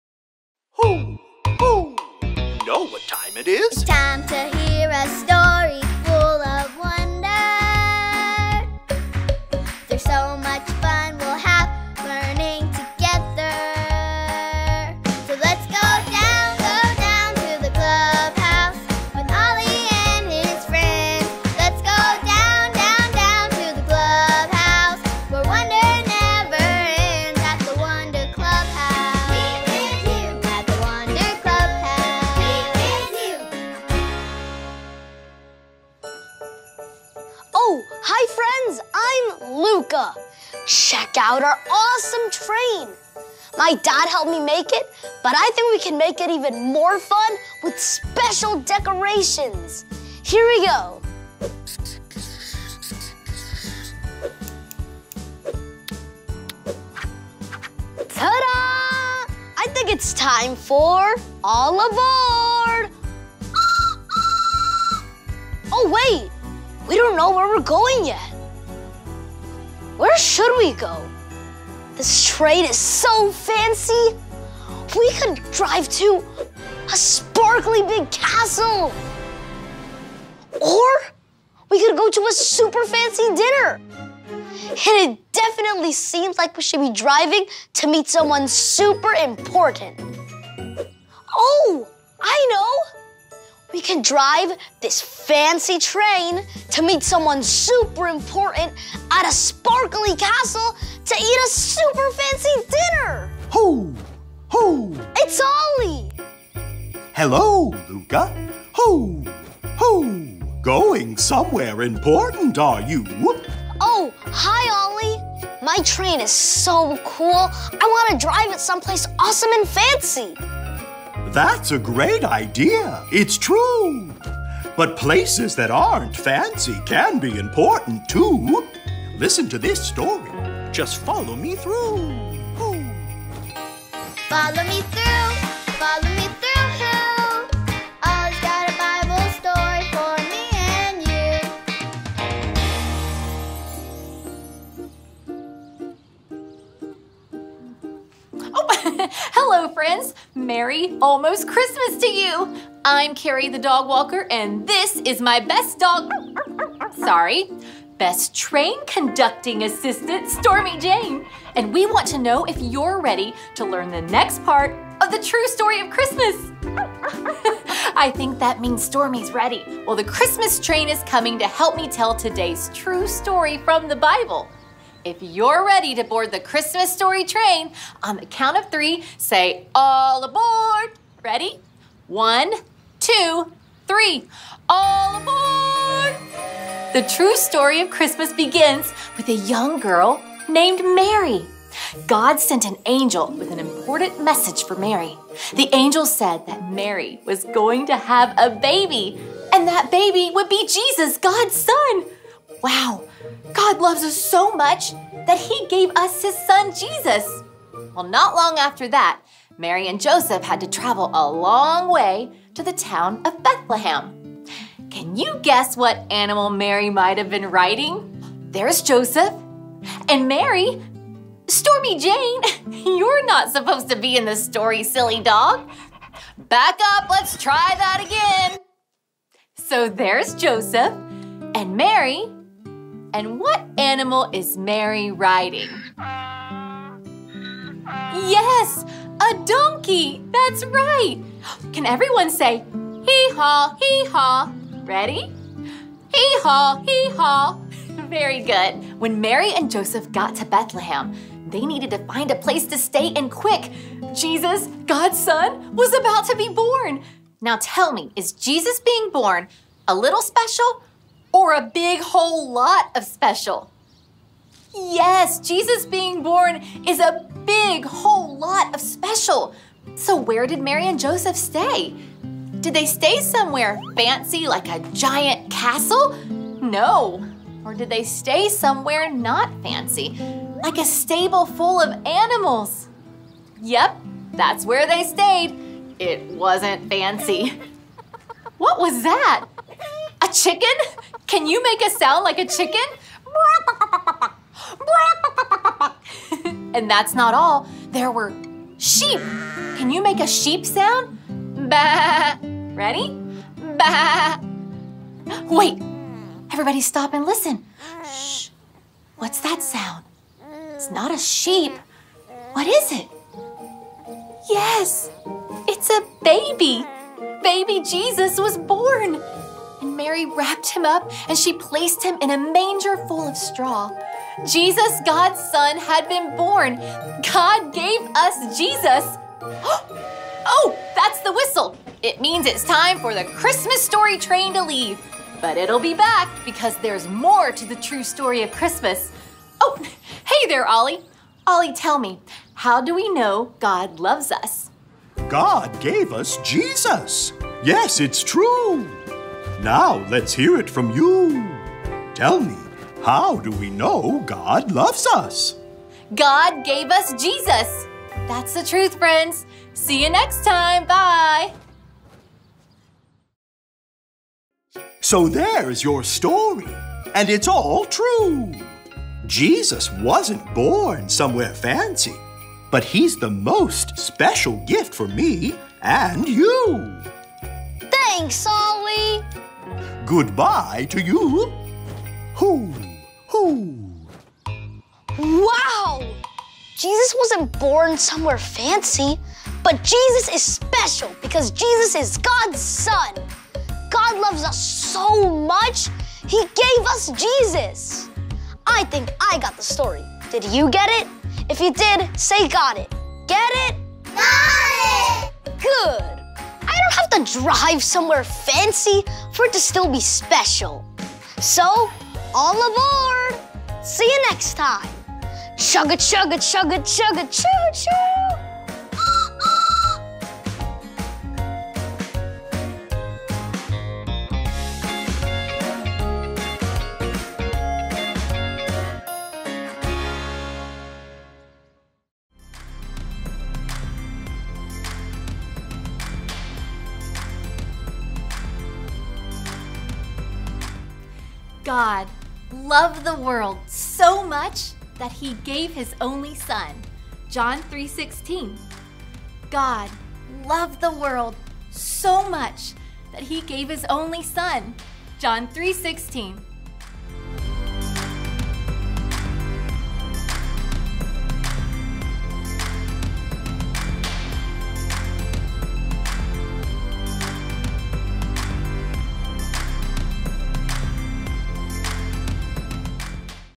A Whole Lot of Special - Jesus Is Born! Pre-K Bible Story
God loves us so much that he gave us his son Jesus! Let's tell everyone about God's amazing gift. Best Christmas present ever!